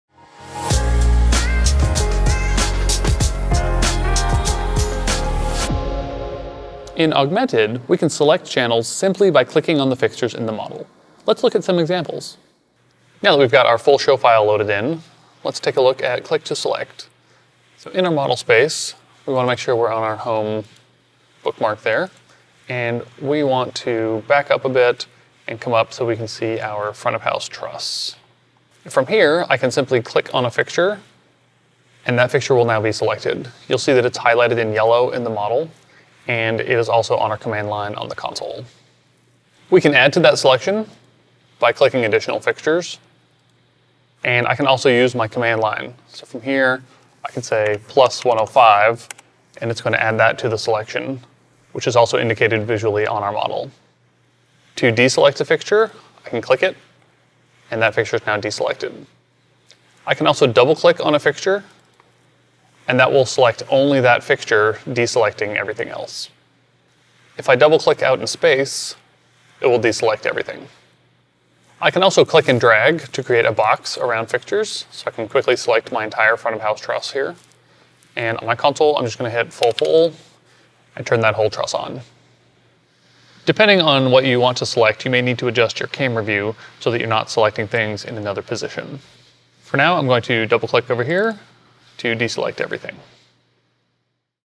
Click to Select
In Augment3d, we can select channels simply by clicking on the fixtures in the model.  These selections are fully interactive with the command line.

For emergency/technical assistance contact ETC Technical Services @:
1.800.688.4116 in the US